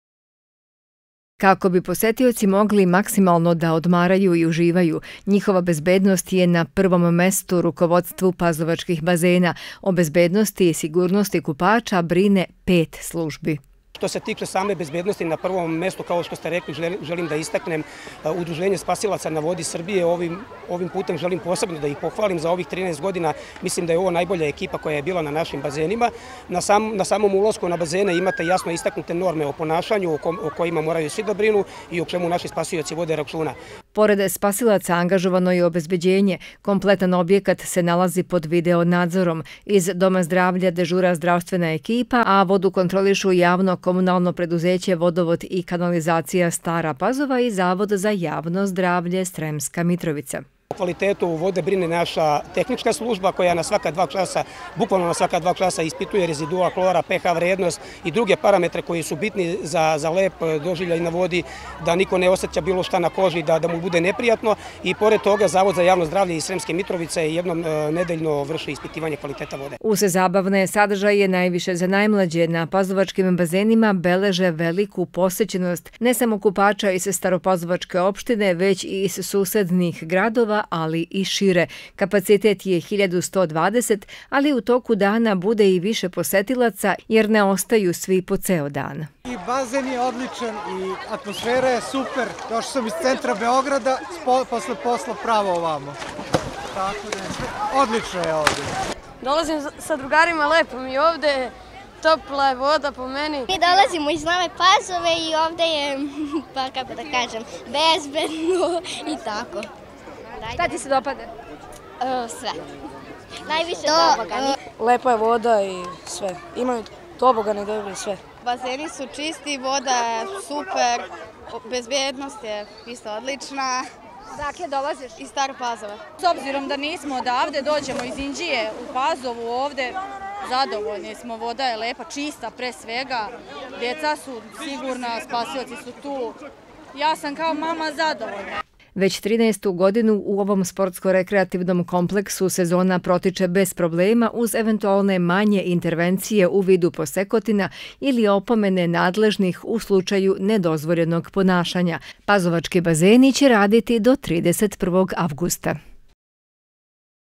Na Pazovačkim bazenima velika posećenost
Kraj leta nam je ponovo doneo vrućine, a odlazak na bazen svakako predstavlja jednu od ommiljenih letnjih aktivnosti,  jer pruža mogućnost odmora, kupanja, plivanja, sunčanja. Ipak da bi boravak na bazenu bio bezbedan, nadležni moraju da vode računa a na Pazovačkim bazenima o bezbednosti kupača brine pet službi. Između ostalog svakodnevno se kontroliše i kvalitet vode.